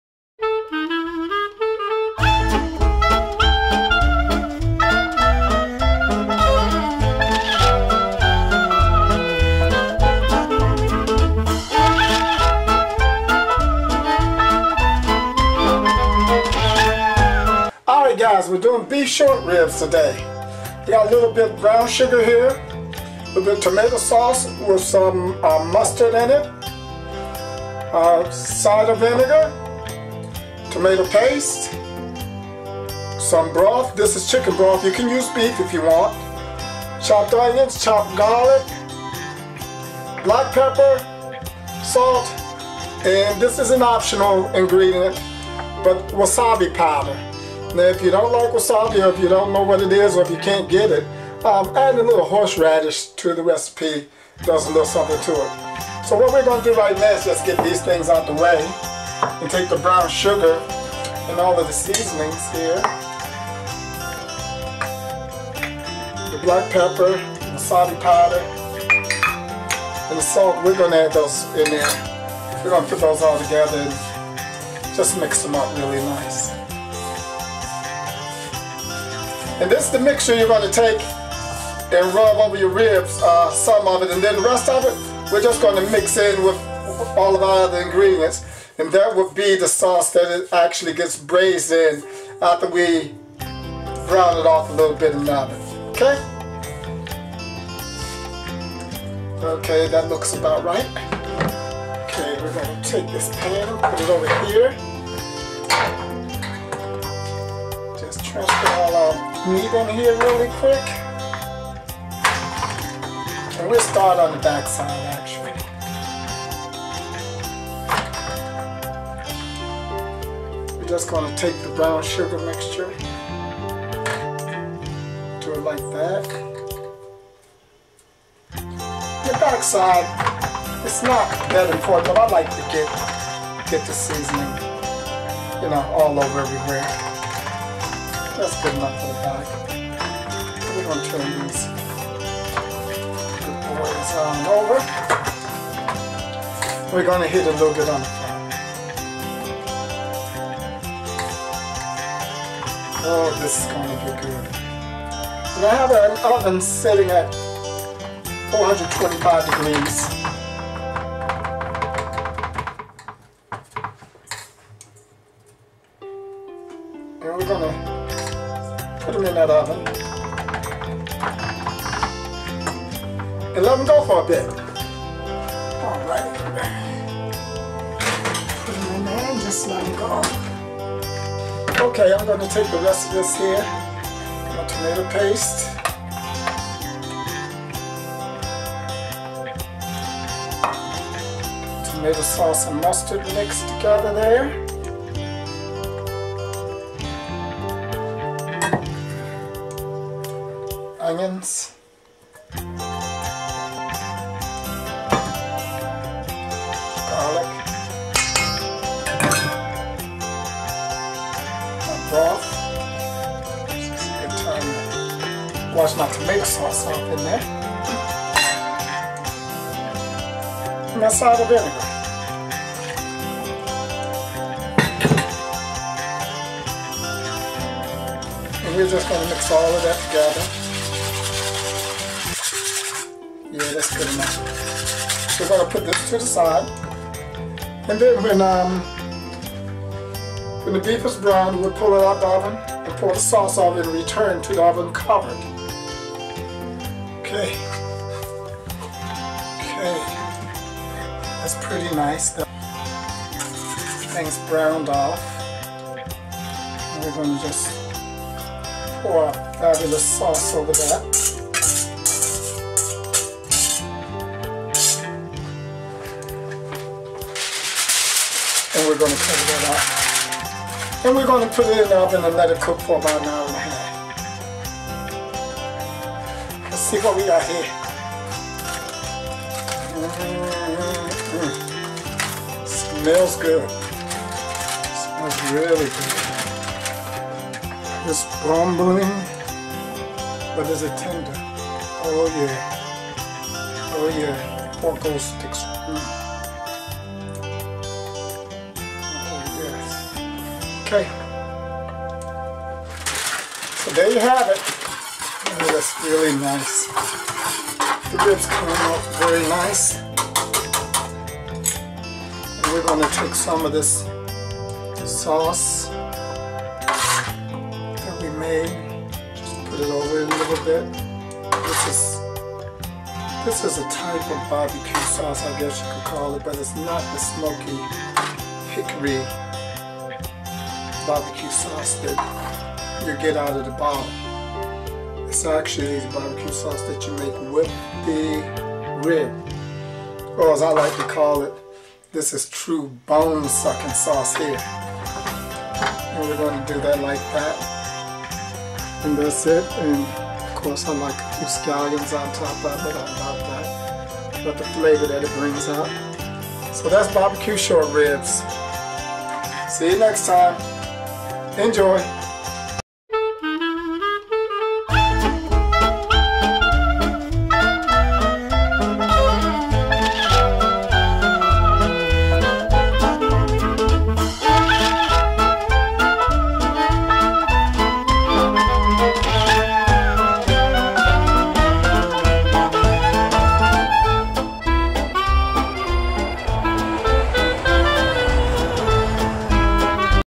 SHORT RIBS BRAISED BBQ
Short ribs of beef bbq is a year round favorite of many people. has it's own take on many food favorites and you can also get recipes for many other items